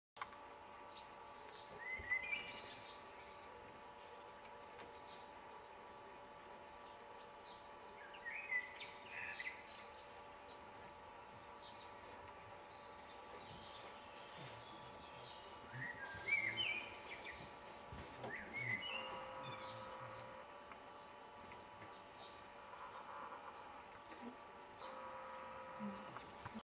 Red Squirrel Rare Wallington
A Red Squirrel which are fairly rare eating some nuts upside down on a tree in Wallington Northumberland